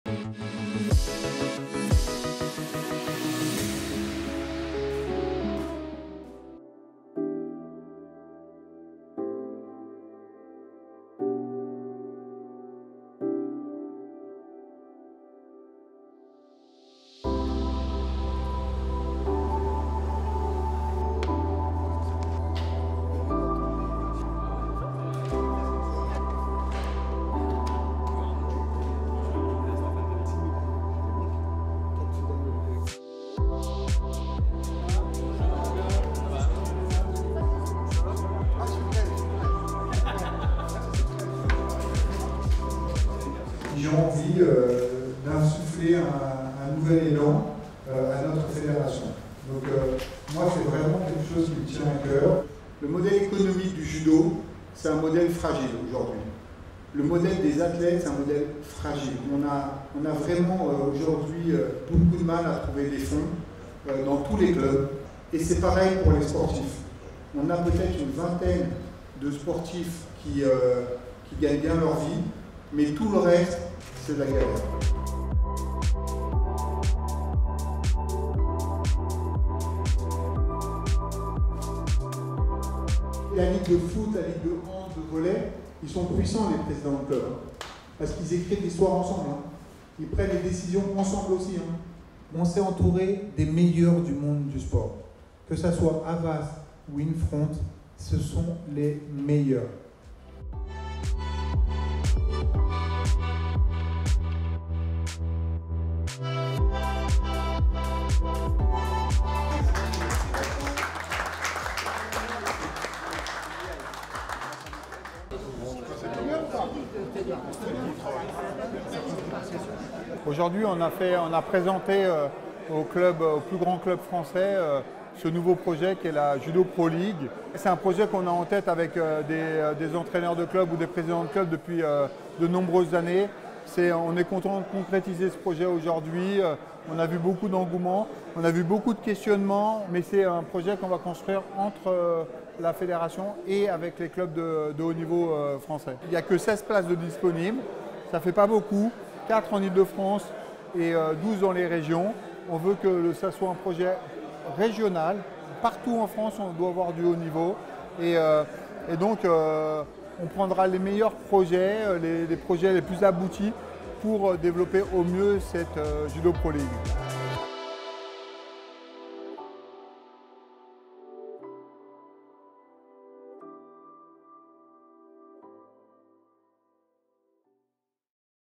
La Judo Pro League : un nouvel élan pour le judo français !
Ce mardi 22 juin, le Président de France Judo, Stéphane Nomis, a invité à la fédération les présidents des clubs de haut niveau ainsi que les présidents de ligue à une réunion de présentation de la Judo Pro League.

Stéphane Nomis était accompagné par la société Havas Play, qui travaille depuis plusieurs mois avec les équipes de France Judo, pour construire ce championnat professionnel.